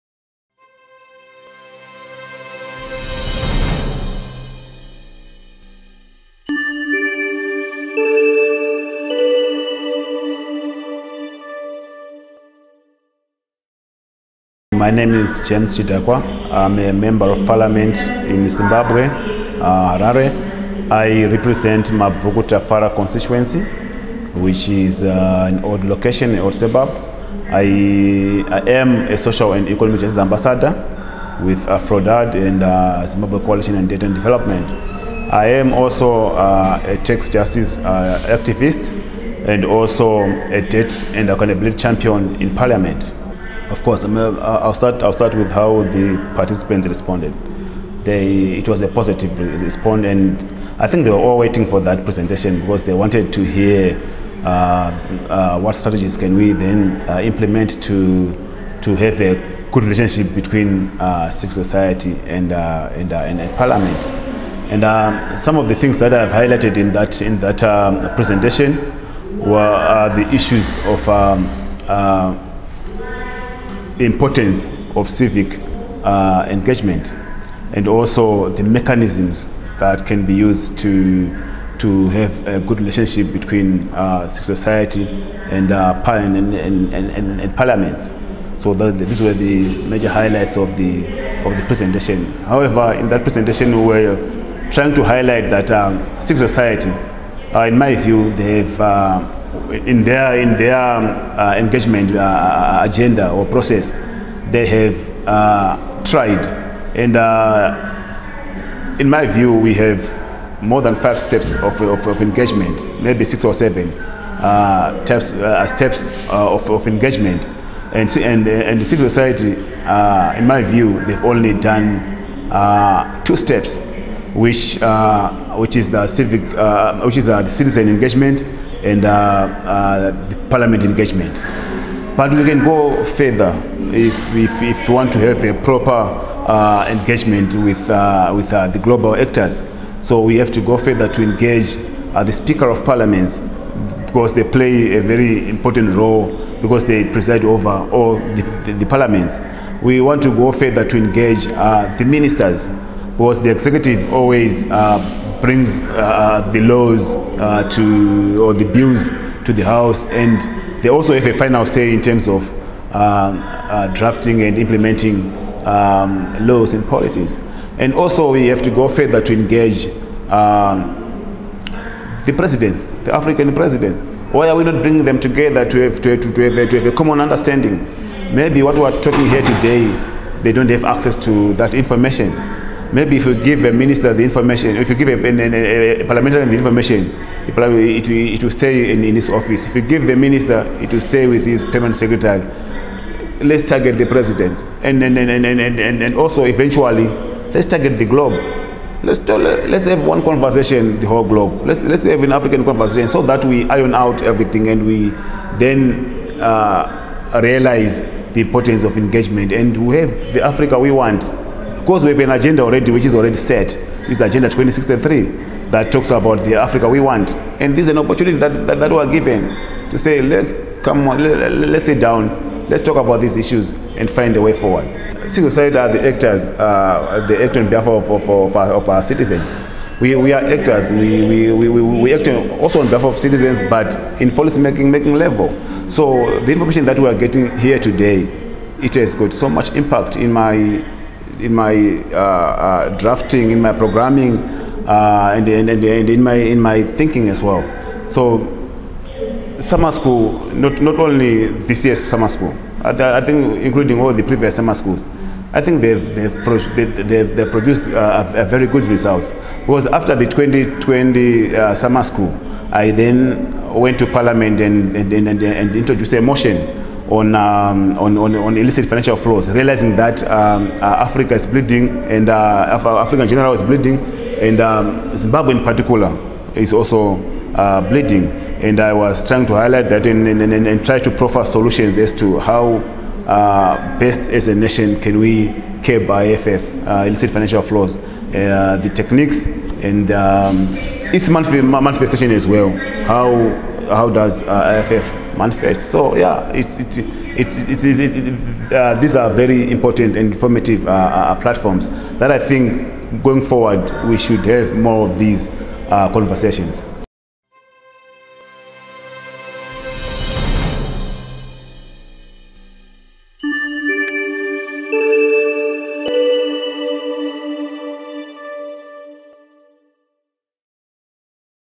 Hon. James Chidakwa- Summer School Interview
Zimbabwean Member of Parliament, Hon. James Chidakwa, speaking during the 2021 Summer School. He talks about his presentation (Strategies for effective civil society engagement with legislators) and emphasizes the importance of engaging African governments to have a common understanding on debt.